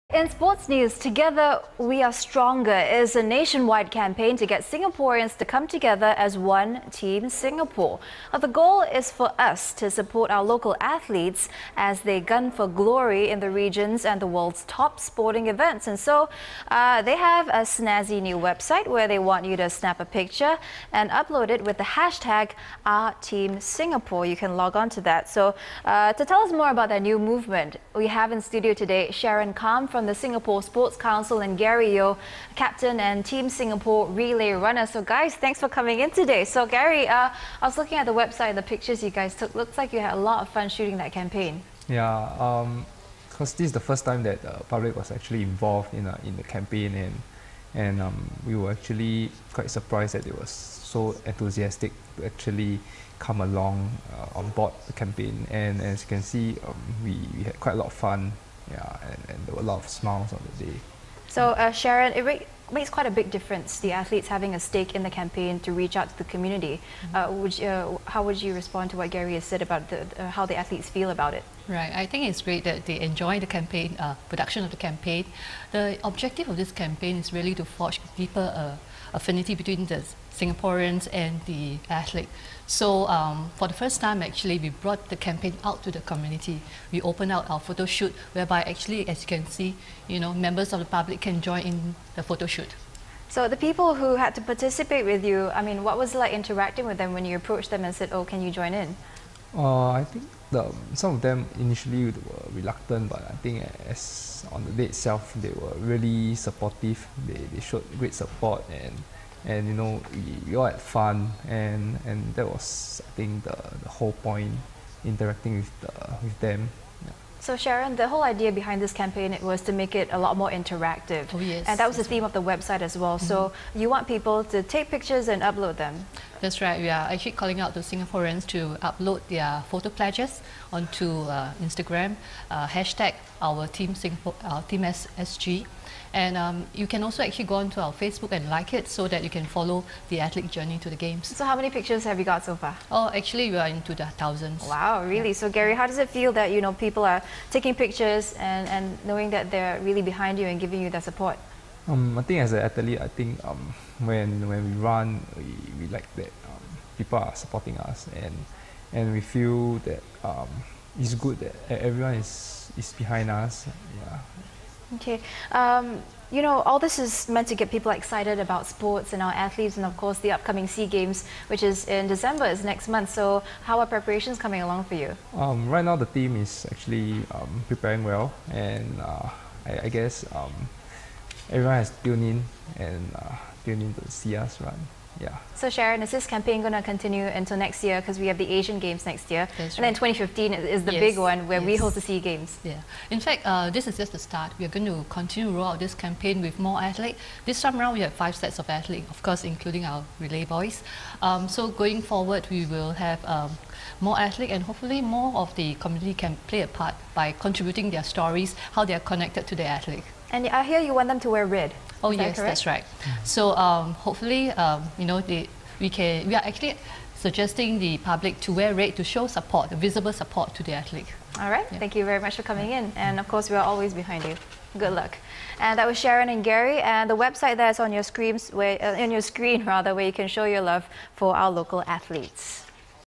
6 Nov 2013: OurTeamSg Interview
Are you behind our athletes as they strive to do us proud?

Sharon Kam from the Singapore Sports Council and Gary Yeo, Captain and Team Singapore Relay Runner, chat with us about getting more people to stand behind Singapore sports.